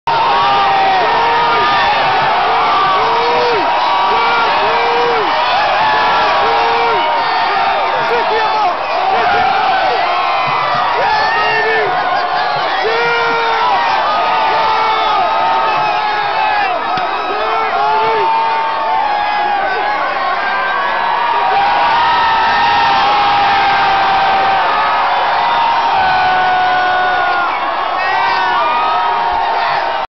Troy Polamalu parade surfing PITTSBURGH STEELERS PARADE
We were sitting 5 rows deep in the crowd when Polamalu decided to jump into the crowd on us. It was the best part of the parade. Thanks for the great year Steelers! Captured by Michael Purvis